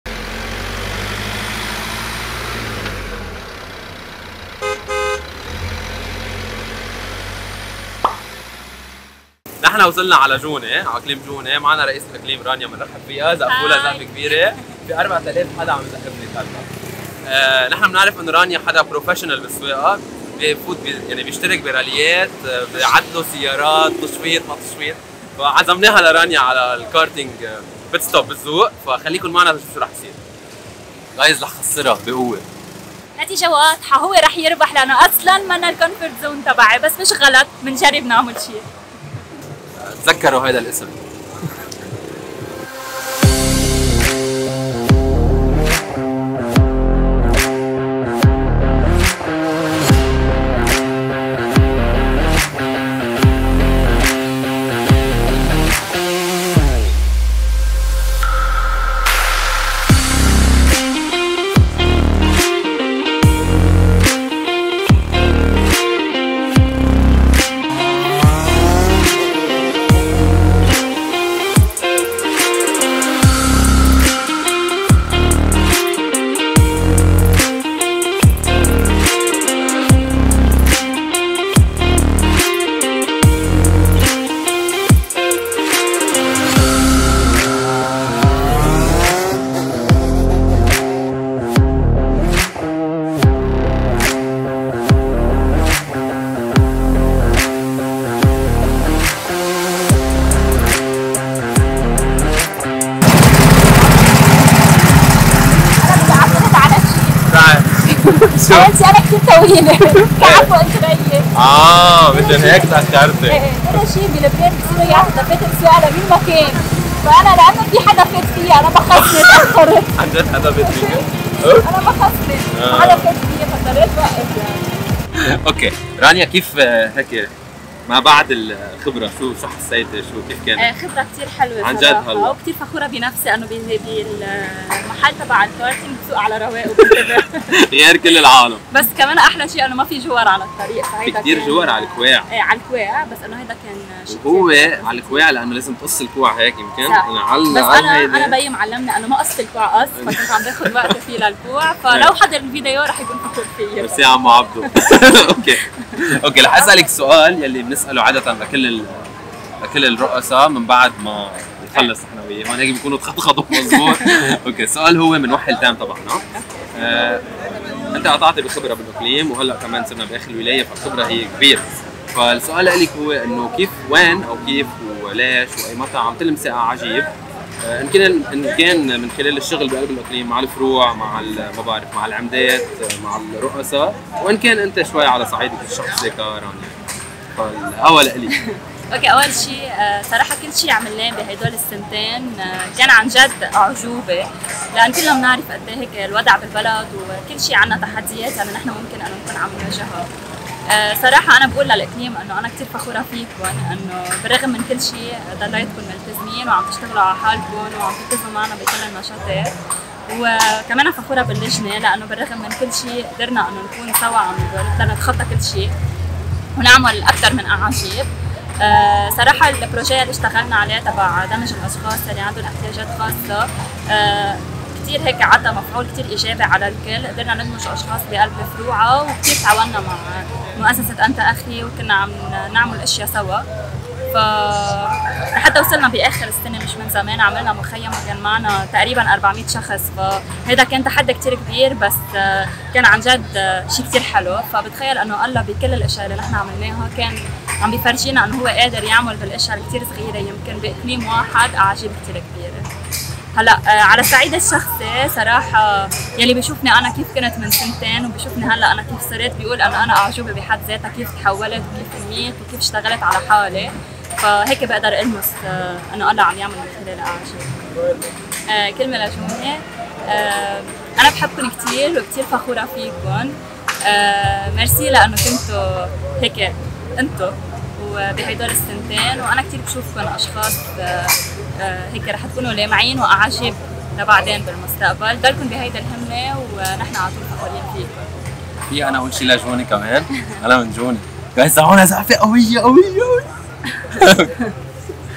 أعاجيب "عالطّريق" ١١ ... مع رانيا طربيه، من إقليم جونية
سلسلة حلقات عفويّة مع رؤساء وممثّلي عن لجان أقاليم أخويّات طلائع العذراء في لبنان...

الحلقة الحادية عشر... رانيا وشربل عم يتسابقوا... من قولكن ربح؟
عطونا آرائكم بالتّعليقات! ⬇️